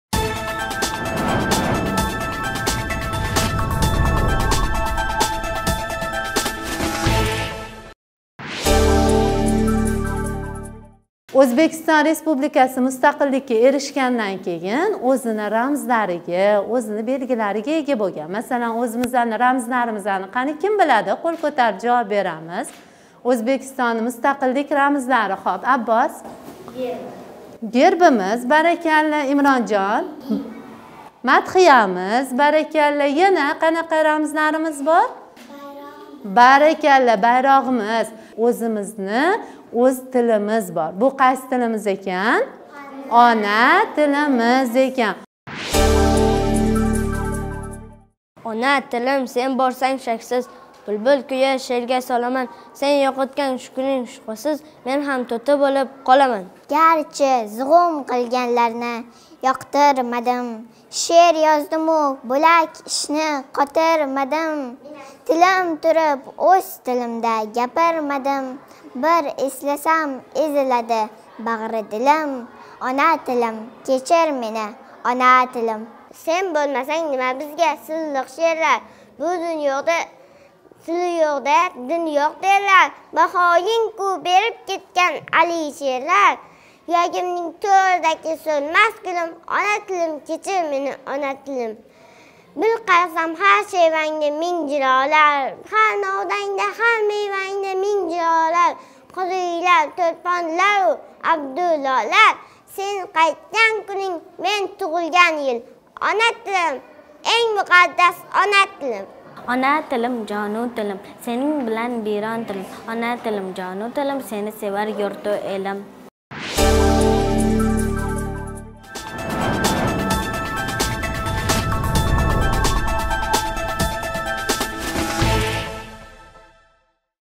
Она тилим — жону дилим
21 октябрь — Ўзбек тилига “Давлат тили” мақоми берилган кун.
Инсон гўзаллиги, маънавияти, буюк мероси, ҳатто олам барқарорлигининг асоси ҳам бу — тилдир. Шу сабаб онадек азиз ўз она тилимизни асраб-авайлаш, эъзозлаш барчамизнинг муқаддас бурчимиз бўлмоғи жоиз.
Айни кунларда тил байрамига доир тадбирлар бутун мамлакатимиз бўйлаб, олий ўқув юртлари, лицей ва мактабларда кўтаринки руҳда бўлиб ўтмоқда. Айниқса келажак авлод вакиллари саналган фарзандларимизга миллий тилни севиш ва асраш руҳини сингдириш, меҳр-муҳаббат уйғотиш ҳар бир инсоннинг зиммасида турган ўта масъулиятли вазифа саналади.
Пойтахтимиздаги мактаблардан бирига ташриф чоғида жажжи ўқувчиларнинг кўзида ёлқинланган оловни, минг ҳаяжон билан “Она тилим — жону дилим” дея баралла айтаётганини, шеърлар ўқиётганини кўриб беҳад севиндик. Ўйлаймизки, олиб борилаётган ишлар уларнинг келажакда ўз тилини асрайдиган, дунёга тараннум этадиган болалар бўлиб вояга етишида катта рол ўйнайди.
Дарҳақиқат, она тилини севмаган одамда ўзи туғилиб ўсган заминга, аждодларимиз минг йиллар давомида яратган буюк маданий меъросга, ота-онага, урф-одатларимизга муҳаббат бўлмайди. Ўзбекистон Республикаси Президенти Шавкат Мирзиёев таъкидлаганидек: “Кимда ким ўзбек тилининг бор латофатини, жозибаси ва таъсир кучини, чексиз имкониятларини ҳис қилмоқчи бўлса, мунис оналаримизнинг аллаларини, минг йиллик достонларимизни, ўлмас мақомларимизни эшитсин, бахши ва ҳофизларимизнинг сеҳрли қўшиқларига қулоқ тутсин”.